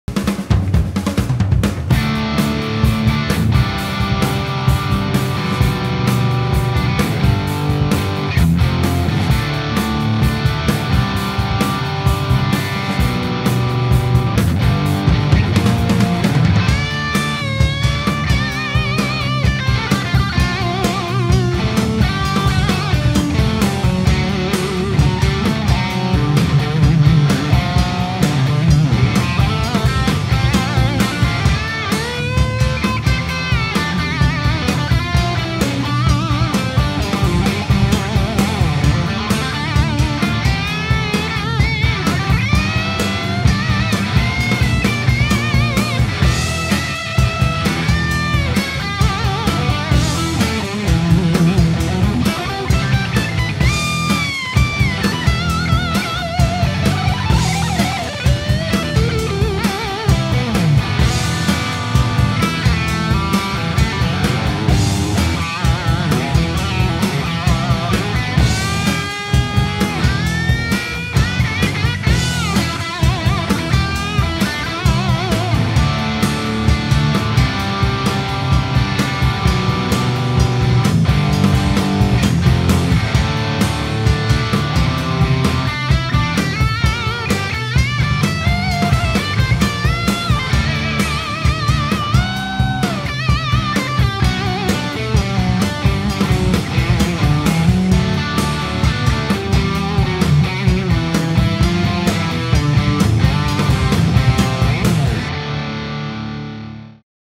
RODENBERG BL800 Lead Series - Overdrive - Take one
RODENBERG British Legend BL800 Lead Series overdrive.
Exact sound copy of the legendary JCM800 with a little more gain (Boost switch).
Rough, direct, british.
• The new FX pedal generation
• British Overdrive Sound
• Built like an amp - sounds like an amp
• All analog signal path
• AVTC – Analog Virtual Tube Circuit
• New analogue high-voltage tube simulation
• MSTB – Mechanical Silent True Bypass
• Internal supply voltage 280V regulated
• Power supply BL800: 9V DC / 250mA

What makes a good amp sound?
Tube technology, selected high-voltage components, dynamics through high-voltage technology and an uncompromising analogue signal path.
The BL800 goes exactly this way. AVTC - Analog Virtual Tube Circuit, selected high-voltage components, 280V operating voltage and completely analog signal path. The BL800 is built like a real tube amp, with the same components, and that's exactly how it sounds!

The BL800 OD works internally with a regulated operating voltage of 280V DC, the external power supply should be a standard 9V DC regulated power supply with 250mA power consumption.

Video by Fabian Ratsak - He plays the BL800 in front of a total clean amplifier